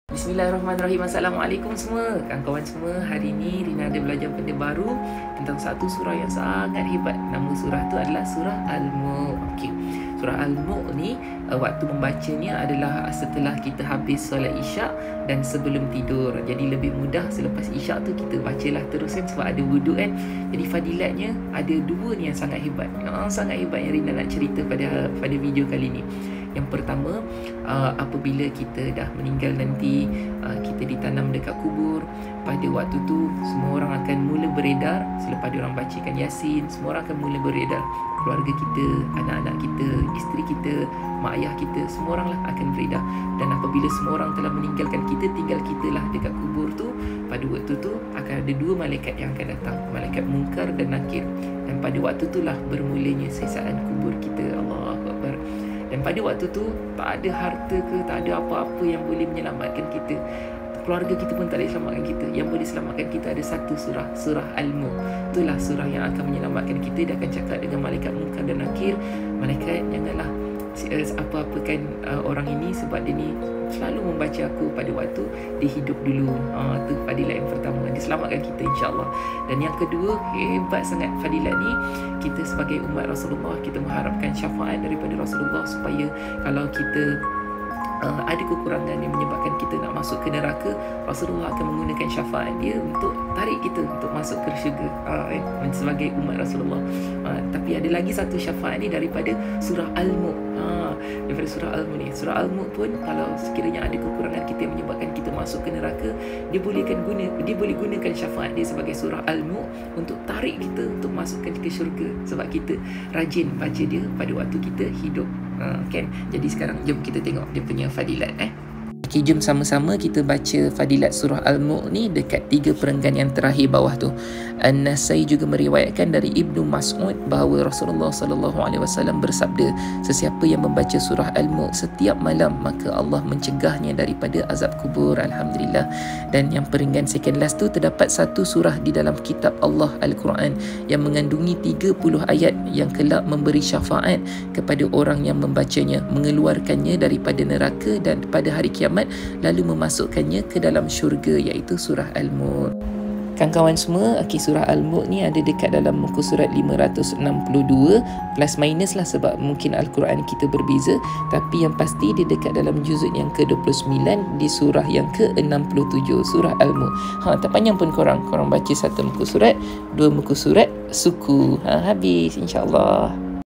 Fadilat Surah Al-Mulk
1. Melindungi kita dari azab kubur
2. Memberi Syafaat & membawa kita keluar dari Neraka dan membawa kita masuk ke Syurga